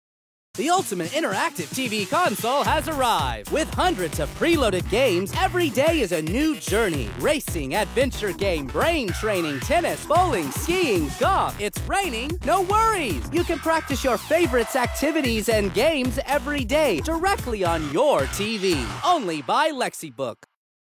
Lexibook Wii Clone Commercial
Yep, the crappy Wii clone that's more like a PC from the 1990s has a commercial.
Downloaded from the Amazon page.